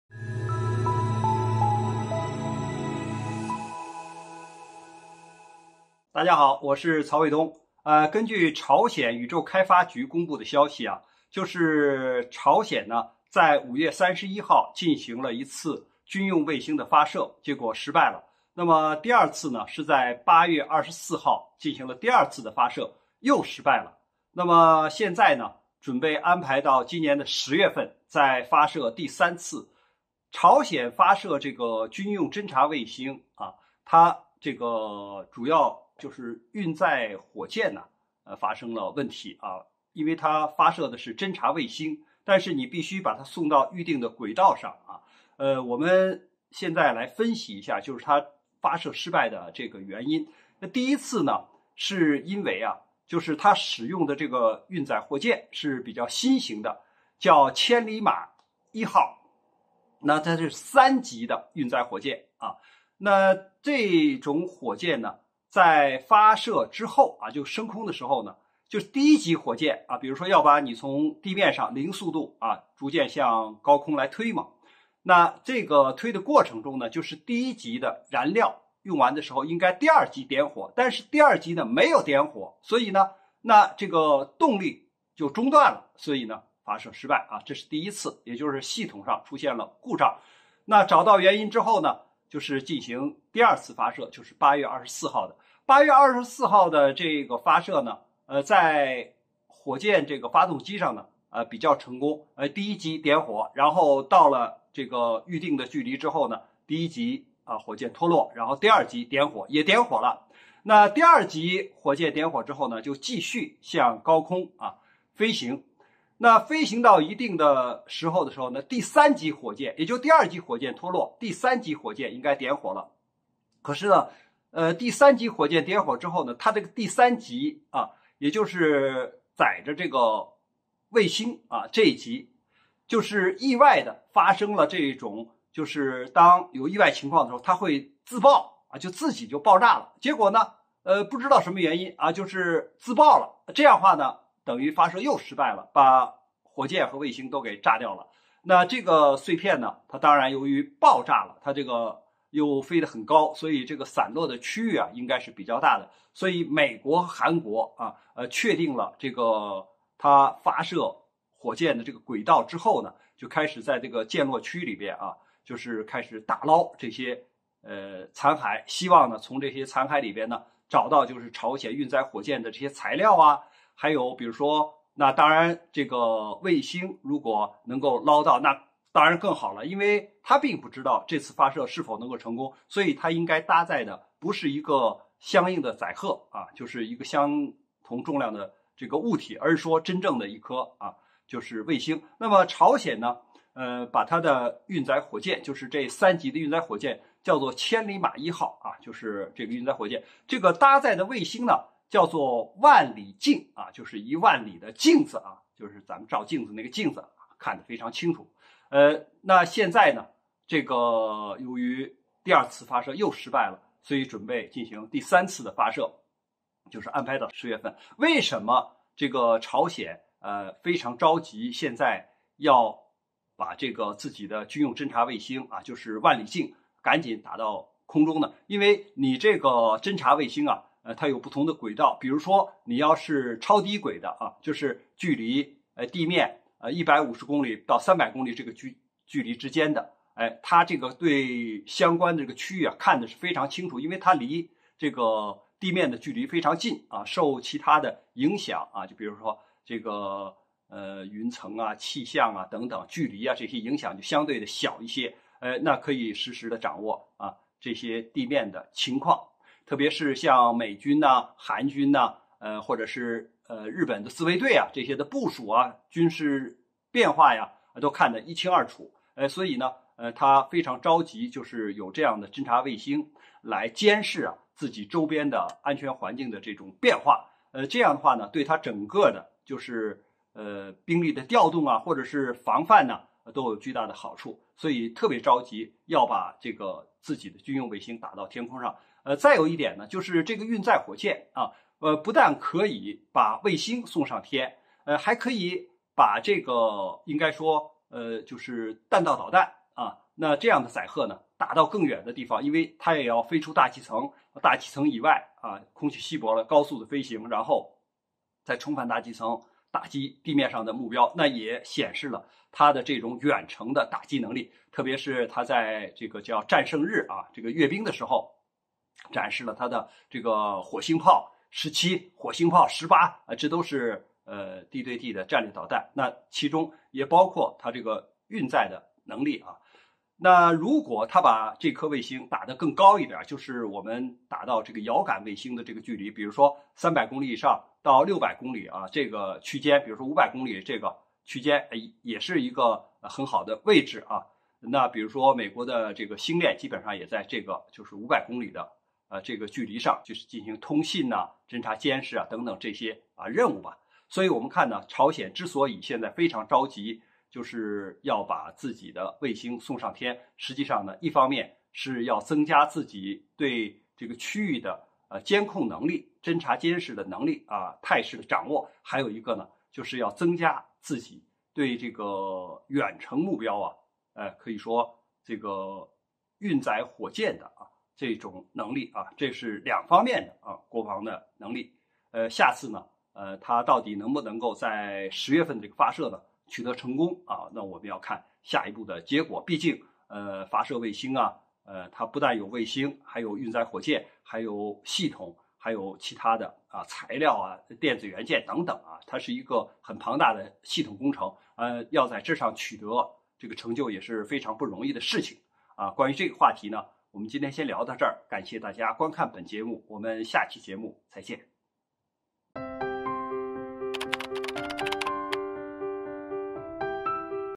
朝鮮第二次發射衛星再失敗，但是技術進步不可小覷，非常不容易【曹卫东】
朝鮮第二次發射衛星再失敗，但是技術進步不可小覷，非常不容易
矚望雲霄# 本頻道製作的所有影片都是為了向觀眾傳遞有趣的社會時事新聞，絕對符合平台的使用政策。